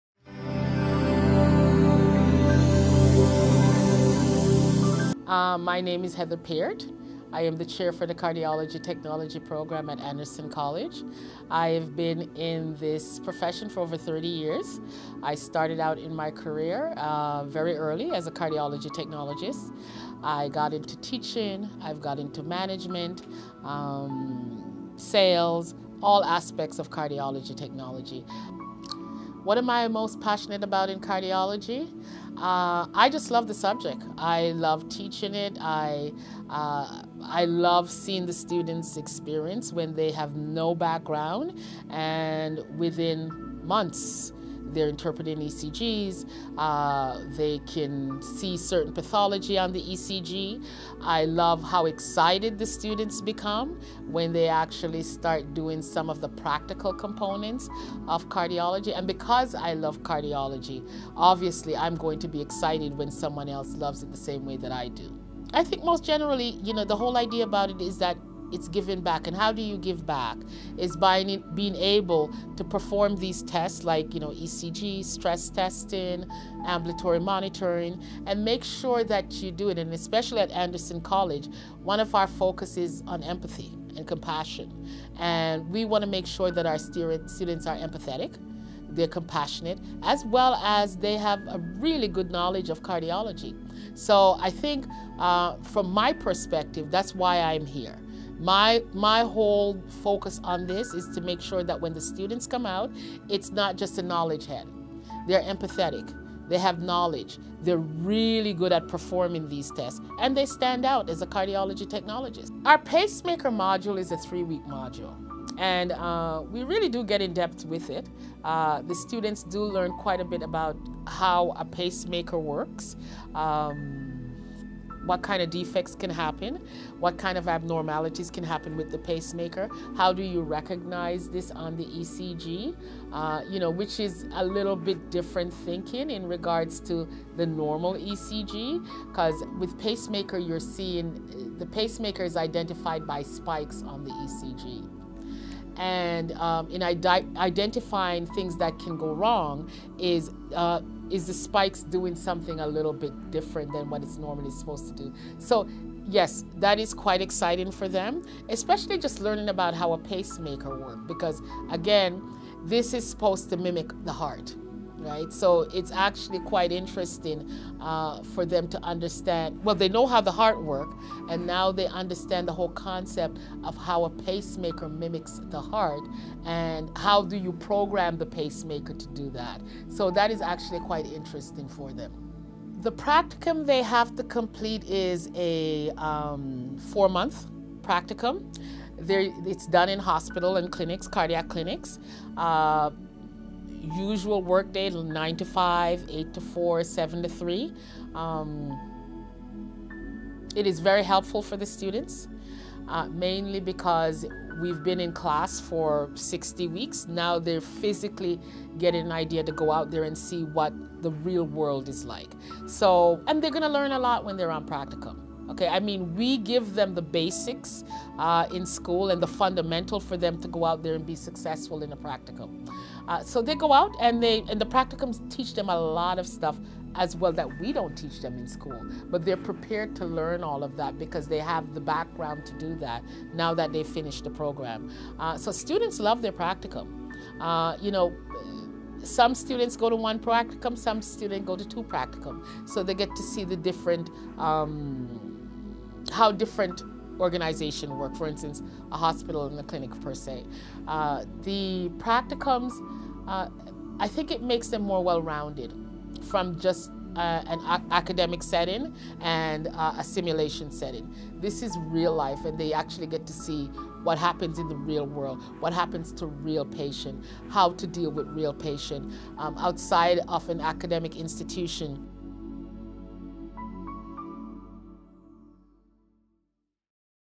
What Will You Learn In The Cardiology Technology Program?
Wondering what you will learn during the Cardiology Technology program? From ECGs to pacemakers, our Program Chair, Heather Peart, tells you what you can expect from this cardiac technology course.